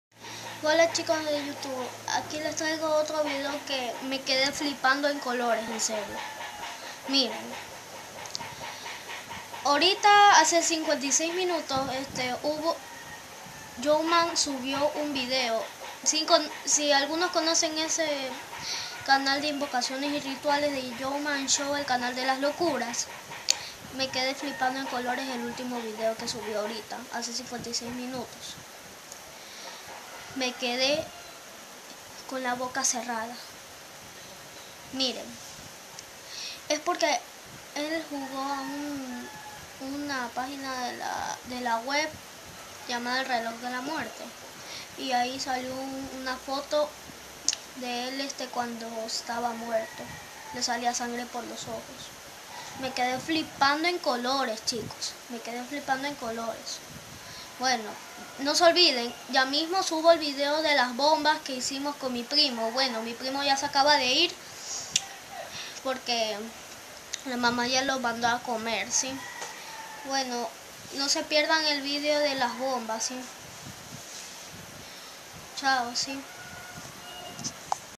Reacción del vídeo de youman del reloj de la muerte.
Hola chicos aquí les traigo un vídeo  que se trata de the youman show por su muerte .